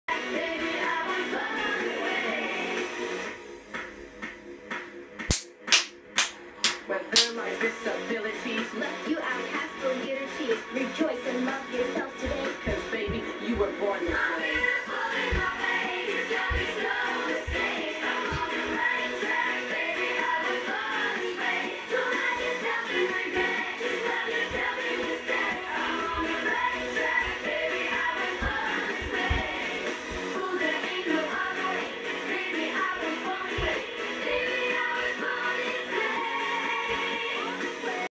Just Pretend Kids' Ladybug Collection Gets Toy Insider Kid Lena in the Groove!
Lena, The Toy Insider Kid, has a great time dancing around in Just Pretend Kids' Ladybug Collection. She obviously plans to wear this outfit all day (and probably for the foreseeable future). :)